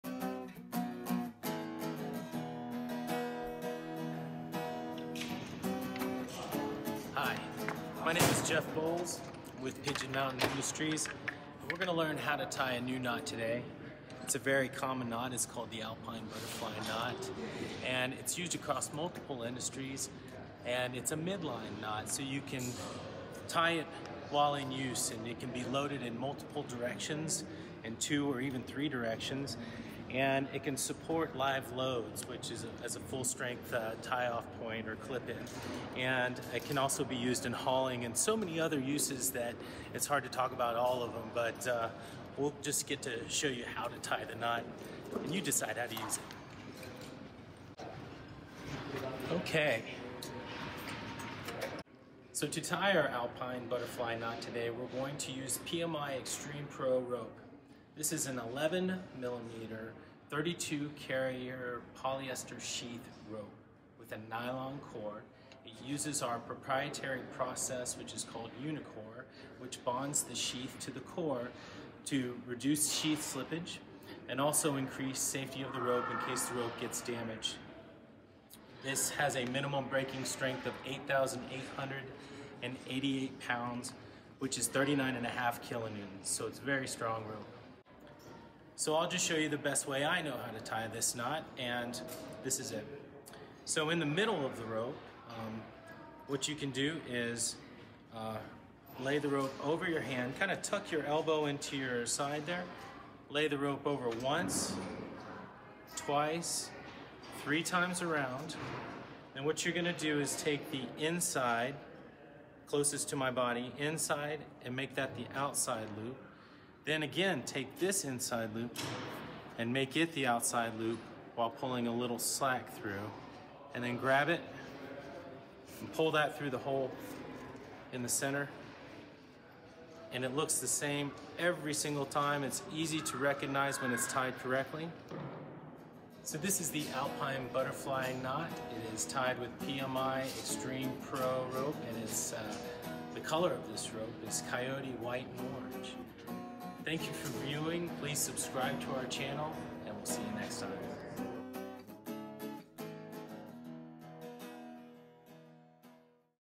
Rope Access: How to tie a Alpine Butterfly Knot
PMI’s  Alpine Butterfly Knot Video will cover a  type of midline knot common to many different industries including rope access, fire rescue, mountain rescue, tower work, rock climbing, and other rope rigging and rescue applications. Jeff Bowles, Technical Business Development Manager at PMI, describes the Extreme Pro Rope and how to tie a butterfly knot.

This video is NOT a substitute for competent instruction and training. High angle rope techniques are inherently dangerous. Special training is required.